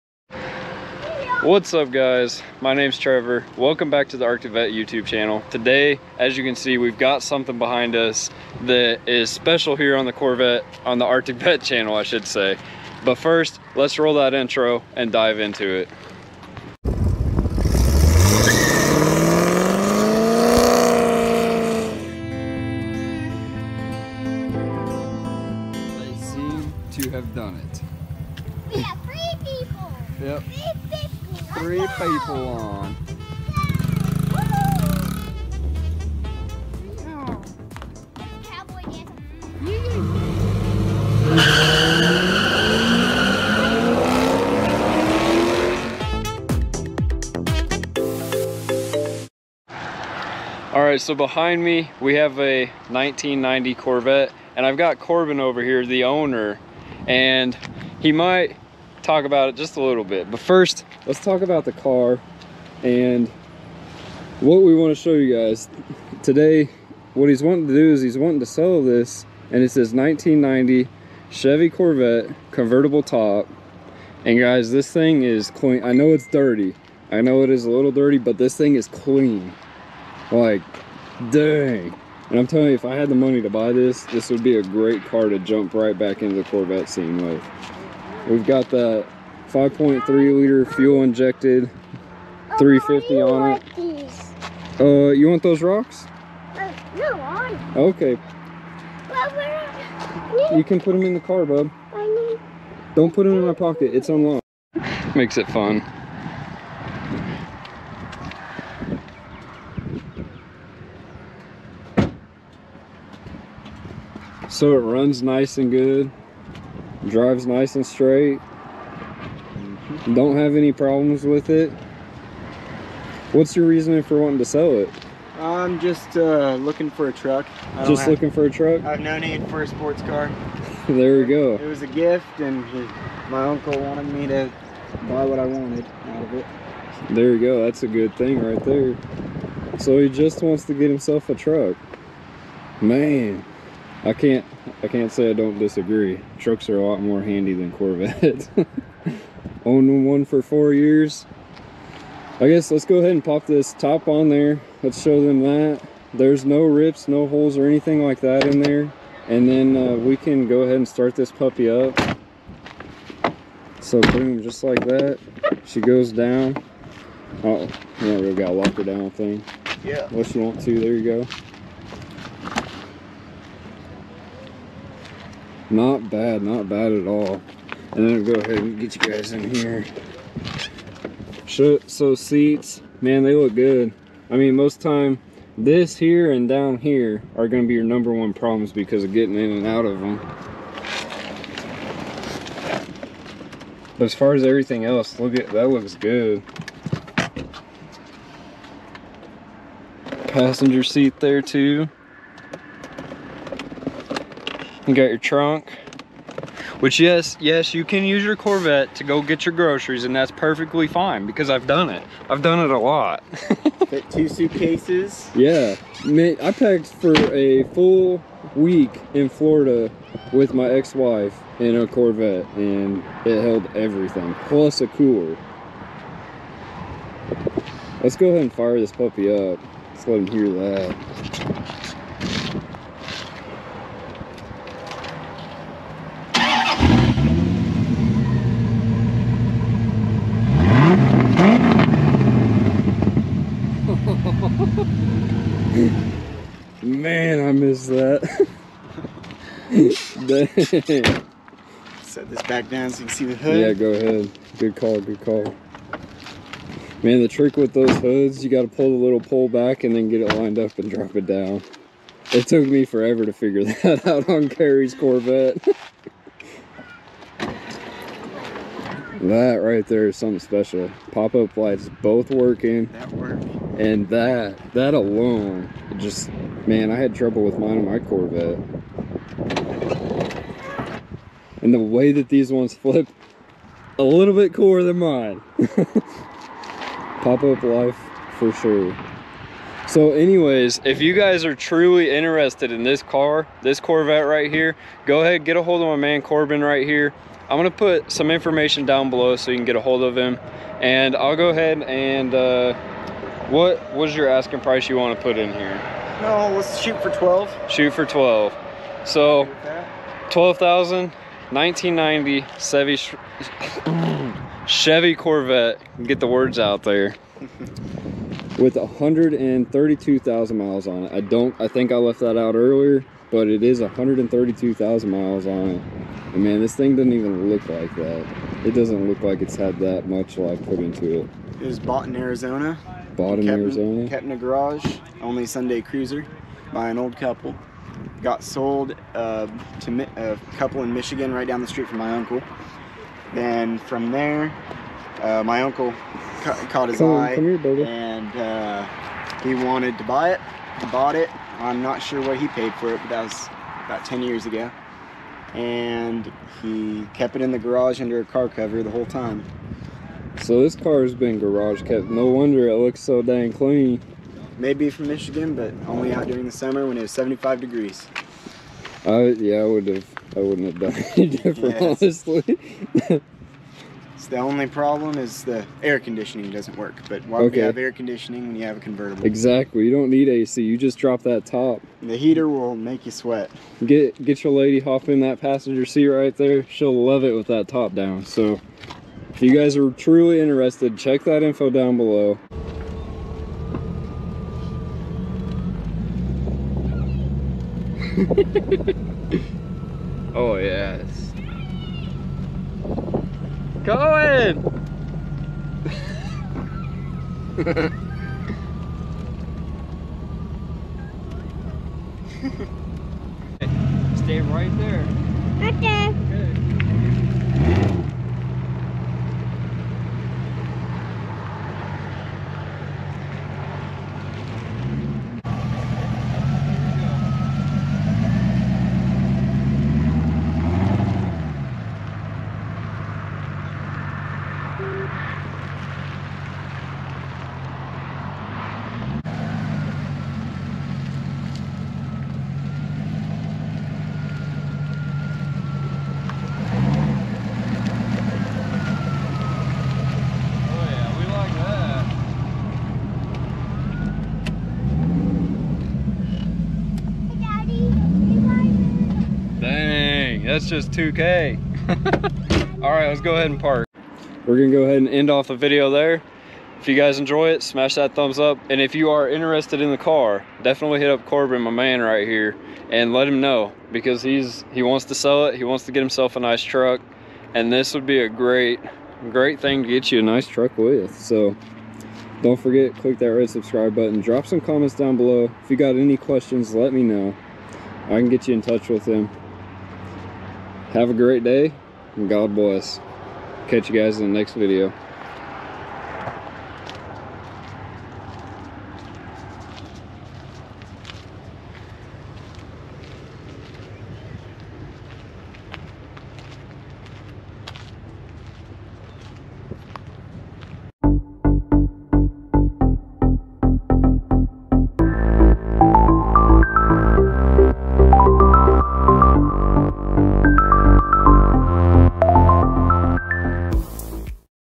Super Clean 1990 Chevy Corvette Convertible For Sale! - Daily Vlog
This is Corbin's 1990 Corvette Convertible with 132k miles, automatic, that he is trying to sell. He is asking $12,000, and price is negotiable.

32gb SD Card (2 pack)